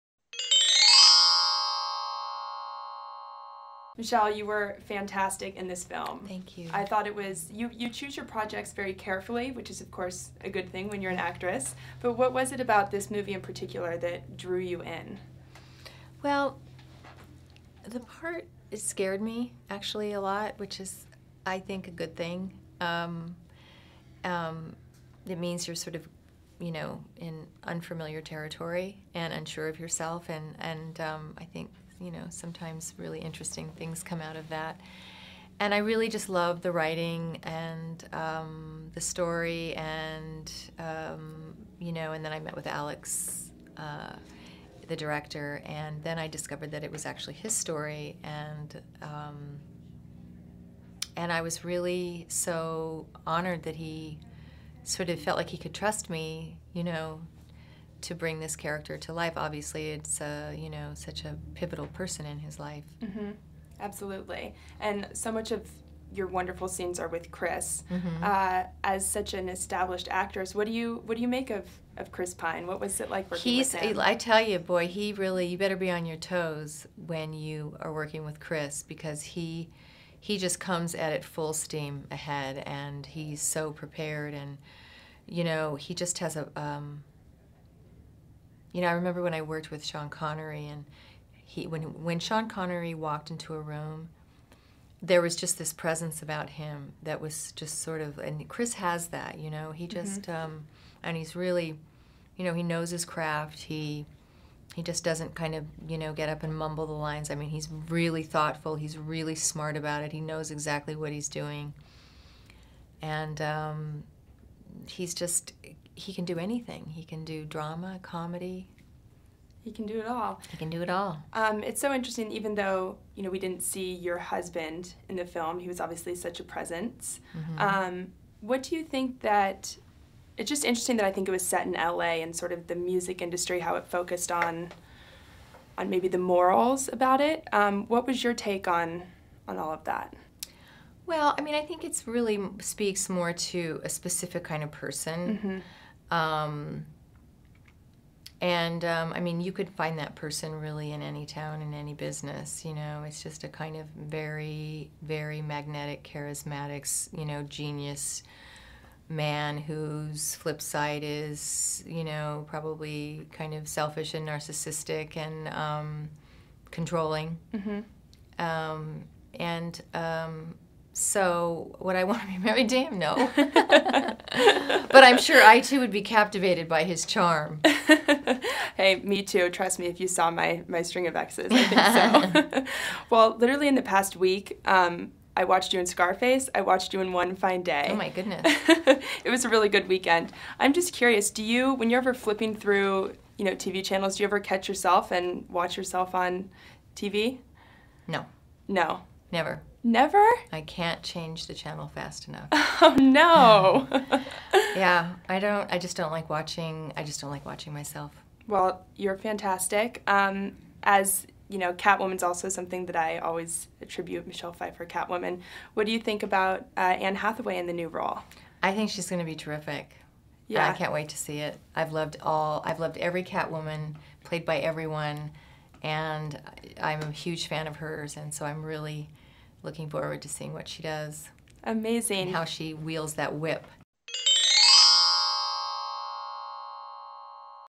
Michelle Pfeiffer on 'People Like Us', Anne Hathaway, and Catwoman
'People Like Us' star talks about her new movie, plus passes on the Catwoman torch